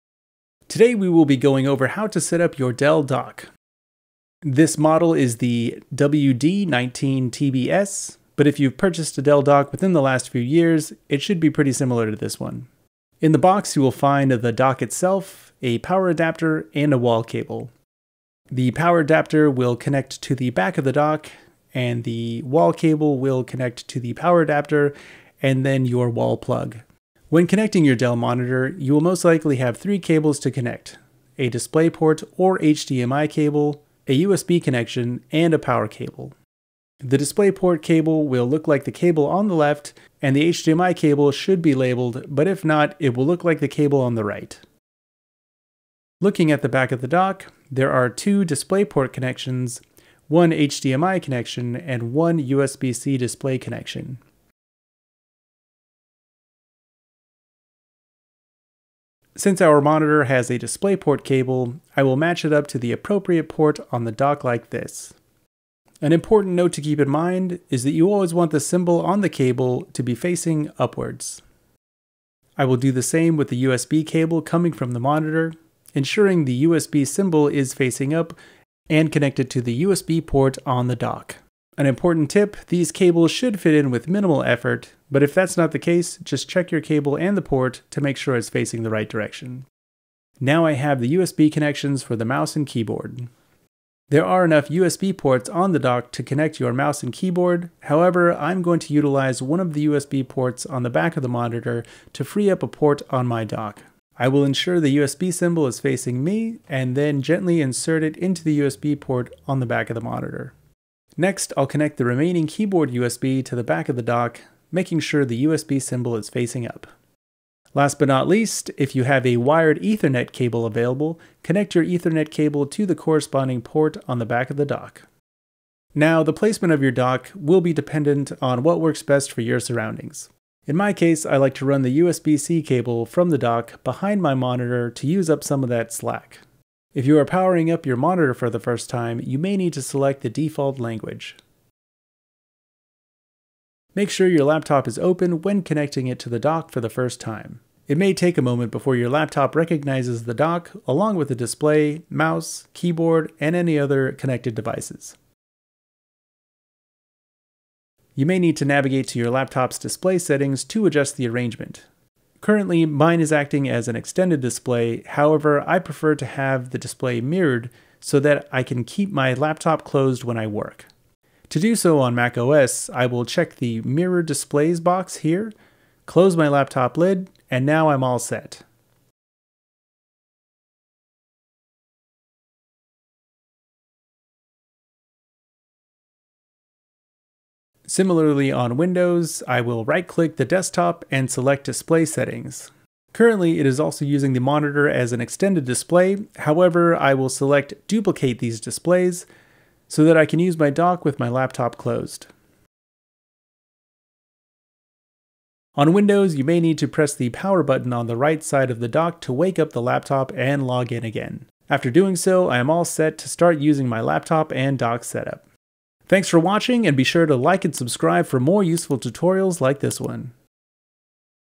Setting Up Your Dell WD19TBS Dock
In this video we go over how to connect your Dell WD19TBS Dock to your monitor, keyboard and mouse.